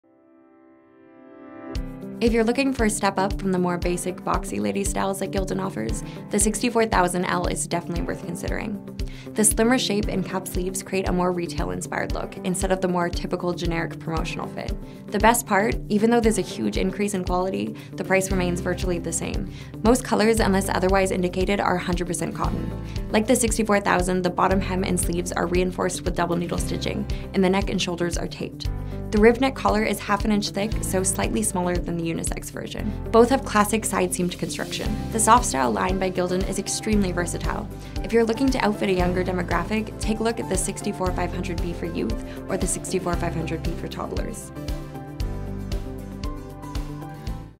Gildan 64000L Ladies' Fitted T-Shirt | Blankapparel.ca
A ladies' tee made for your curves! The Gildan 64000L Ladies' Fitted T-Shirt is a budget fitted t-shirt meant for comfort and style. Watch this video to learn more!